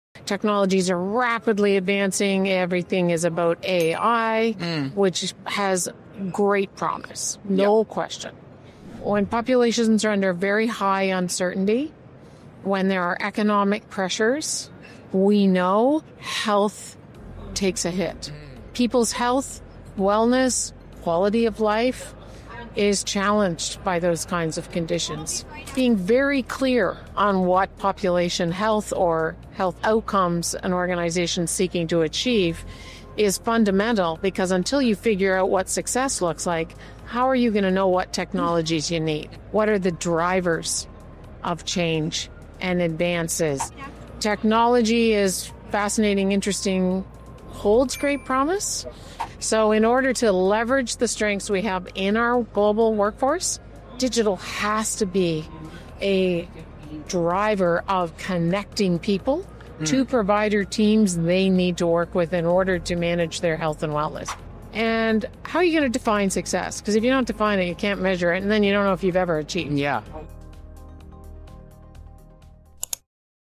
WHAT ARE WE SOLVING?
“In times of uncertainty and economic pressure, health always takes a hit”

Dr. Anne Snowdon from says the promise of AI is real, but without a clear vision of what success looks like in healthcare, tech alone won't move the needle. 🎯💡

This episode was recorded live at HIMSS 2025, where digital health experts from around the globe gather to explore the newest innovations in healthtech.

Tune into episode 517 of the Talking HealthTech podcast on your favourite platform for the full conversation.